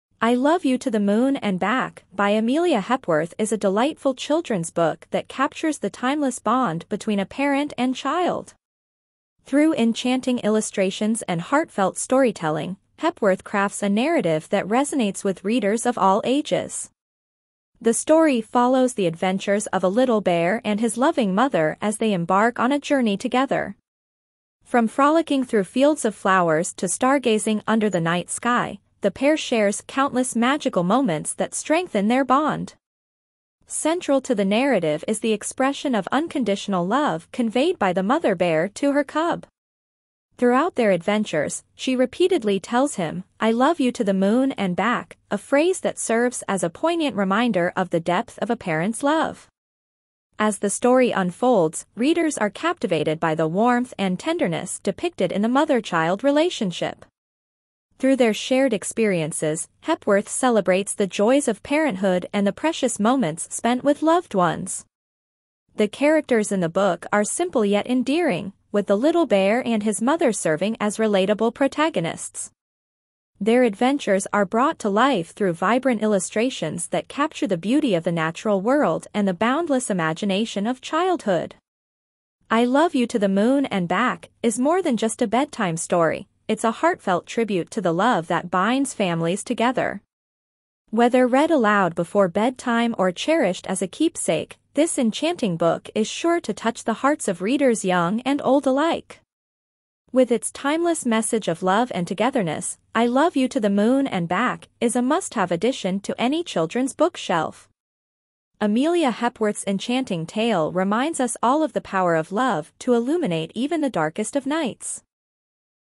Heartwarming Storytime: I Love You to the Moon and Back | Children's Book Summary
oin us for a heartwarming journey with "I Love You to the Moon and Back" by Amelia Hepworth. Discover the magic of this beloved children's book as we delve into the tender bond between a parent and child. With enchanting illustrations and a timeless message of love, this summary is perfect for bedtime story lovers of all ages. Let's explore the power of unconditional love together in this captivating tale.